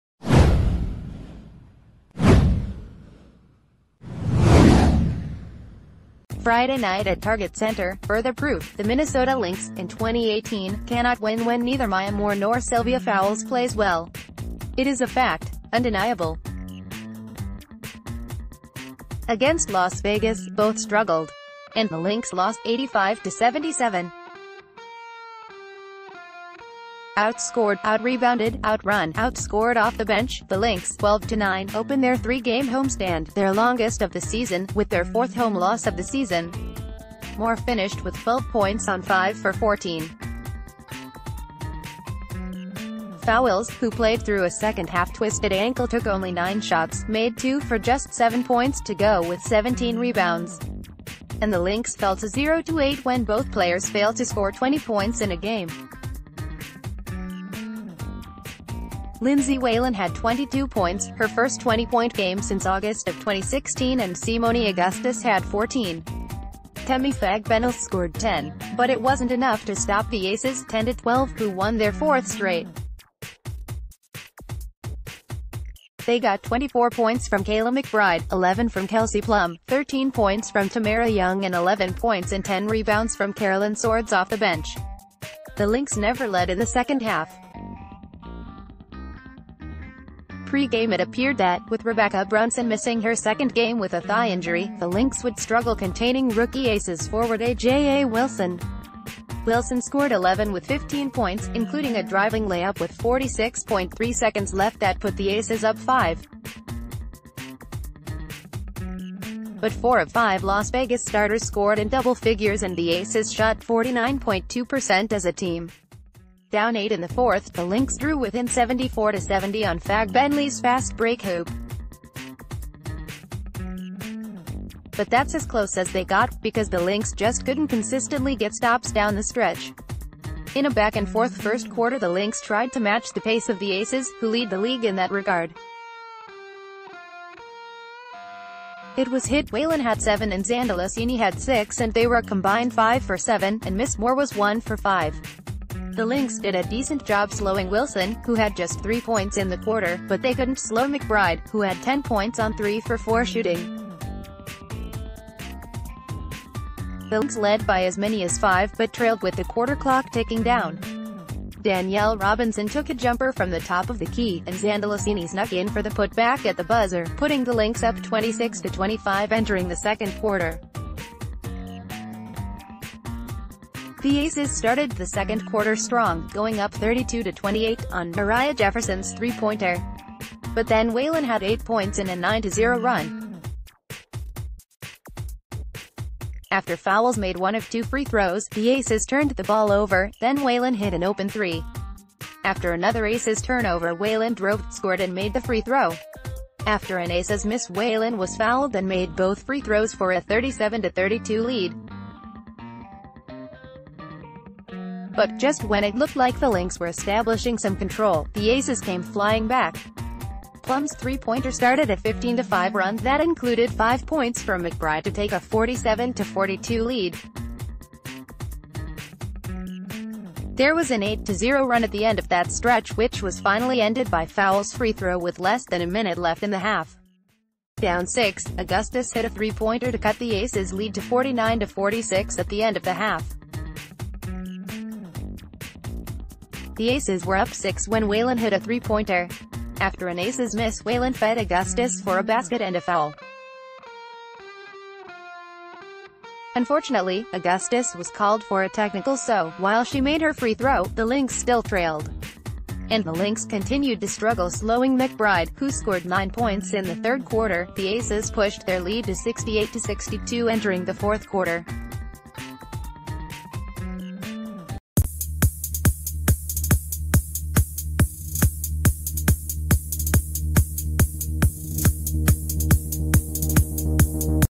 Lynx open homestand with loss to Las Vegas
Friday night at Target Center, further proof: The Minnesota Lynx, in 2018, cannot win when neither Maya Moore nor Sylvia Fowles plays well. It is a fact. Undeniable. Against Las Vegas, both struggled. And the Lynx lost, 85-77. Out-scored, out-rebounded, out-run, out-scored off the bench, the Lynx (12-9) opened their three-game homestand – their longest of the season – with their fourth home loss of the season. Moore finished with 12 points on 5-for-14. Fowles, who played through a second-half tw...